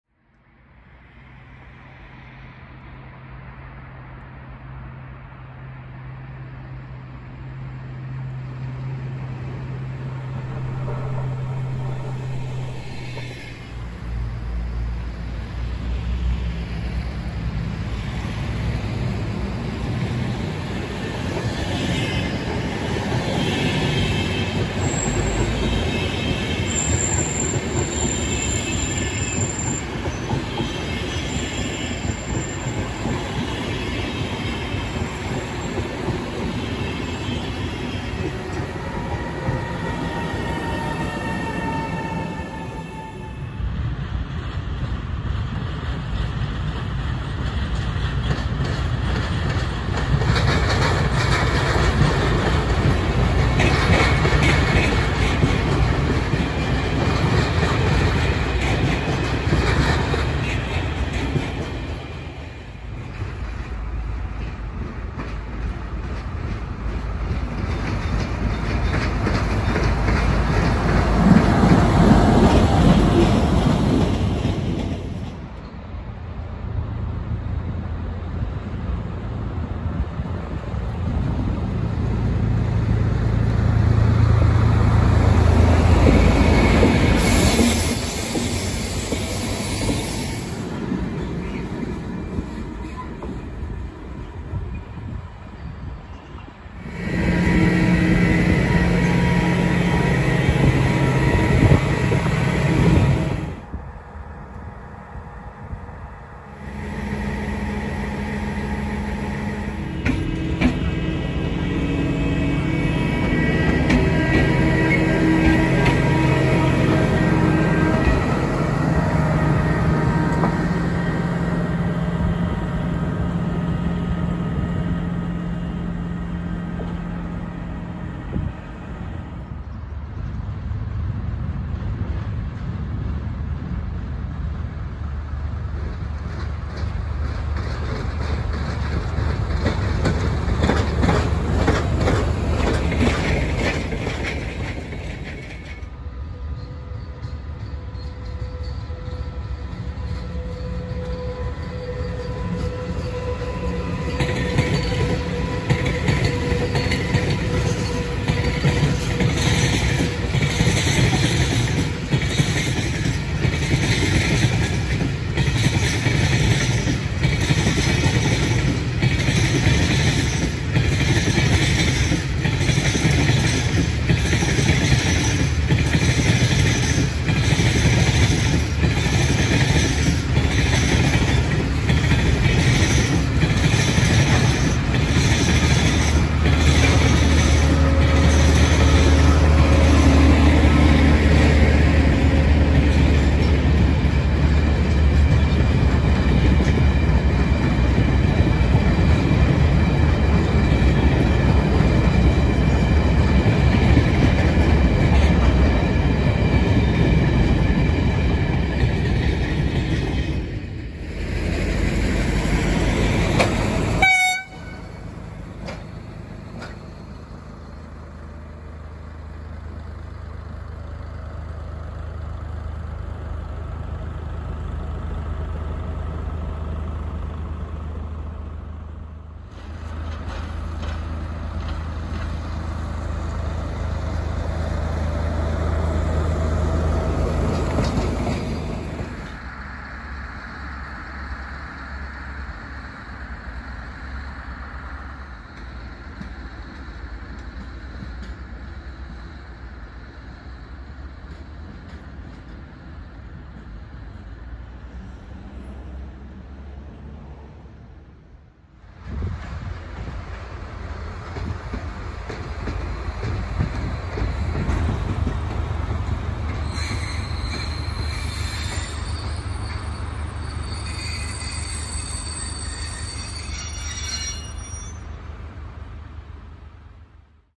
JUNIA VETUREITA TYÖKONEITA TRAINS LOCOS TRACK MACHINES KOKKOLA CITY MARRASKUU 4-9 NOVEMBER 2022
A compilation of various trains, locos and track work machines in the rail yard of my home town, Kokkola City (FIN).